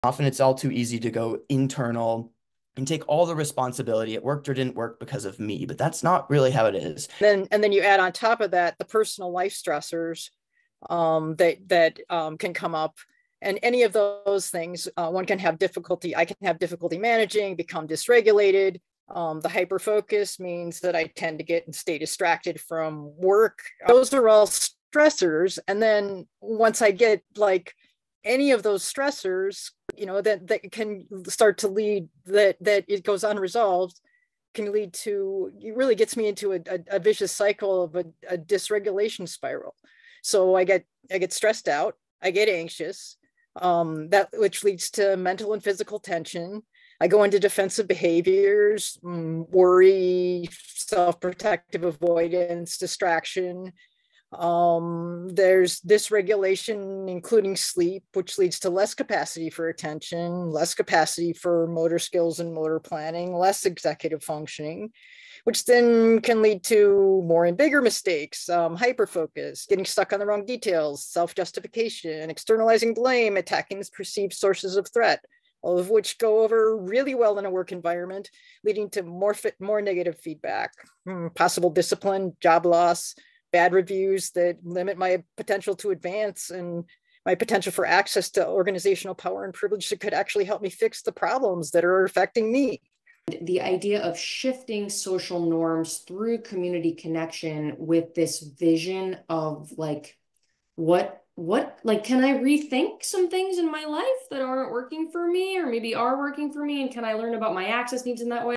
Performance Problems at Work Often Relate to Unmet Access Needs (All Brains Belong VT)
All too often, folks who struggle with employment are told that it's their fault -- when in reality, this is often a mismatch between the environment and someone's access needs. Free registration to access the full episode of Brain Club "Redefining Your Relationship to Work" (2/20/2024)